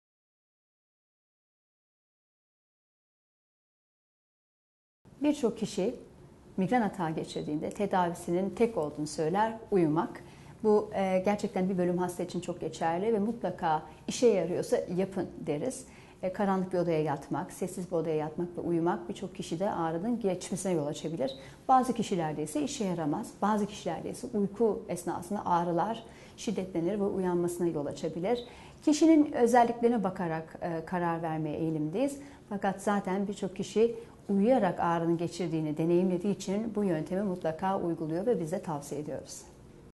Uyumak migren atağını hafifletebilir mi?
Acıbadem Maslak Hastanesi, Nöroloji Uzmanı Dr. Elif Ilgaz Aydınlar, Nörolojik Hastalıklar üzerine soruları yanıtlıyor;